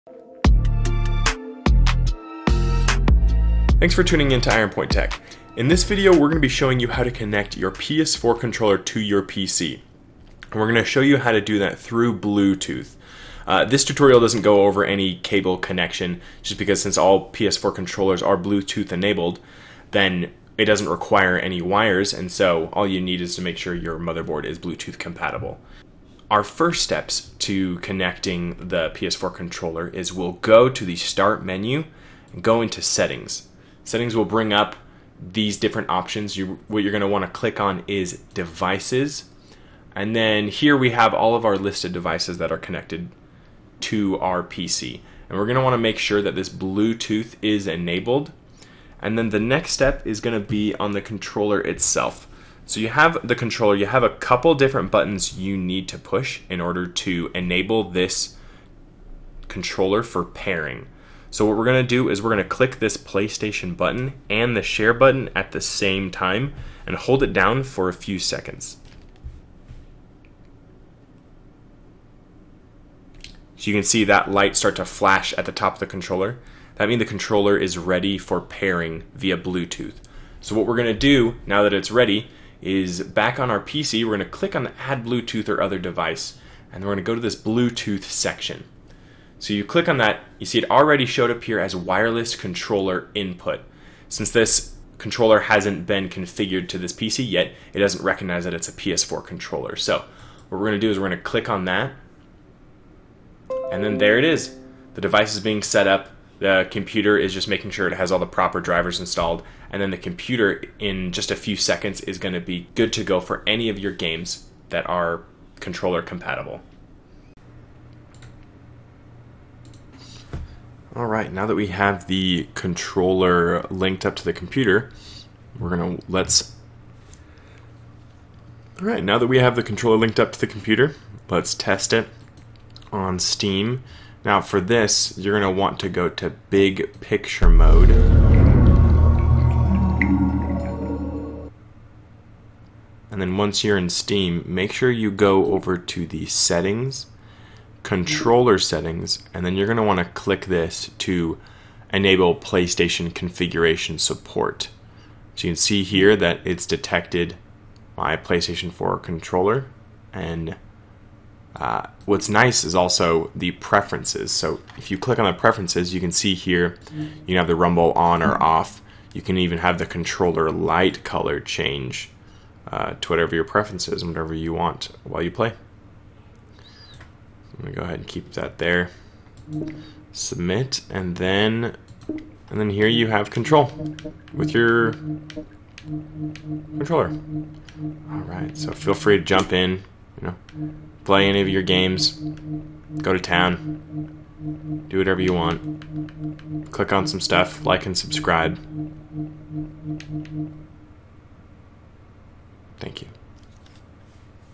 How to Connect PS4 Controller to PC [Easy Method]
To connect your PS4 controller to your PC. You'll need to turn on the controller and have it availably for bluetooth syncing. Then you'll need to go into the settings of your PC.

In this video, I outline the steps of how to connect your PS4 controller to your PC in a few simple steps.

On your PS4 controller, you'll need to hold your PS button and the share button down for a few seconds, until your controller starts flashing white. Then your controller is ready to be synced to your PC via bluetooth. The next step is going into your settings, clicking on your devices and then clicking on add bluetooth devices.

In that menu you should be able to see your PS4 controller and click on it to sync it to the PC.

After the controller is wirelessly connected to your PC. You need to make sure that the settings in any of your games enable your PS4 controller to be used. In Steam, I had to go to the settings to enable PS4 configuration.

Now you've connected your wireless PS4 controller to your PC. If there are easier steps, please drop them in the comments below.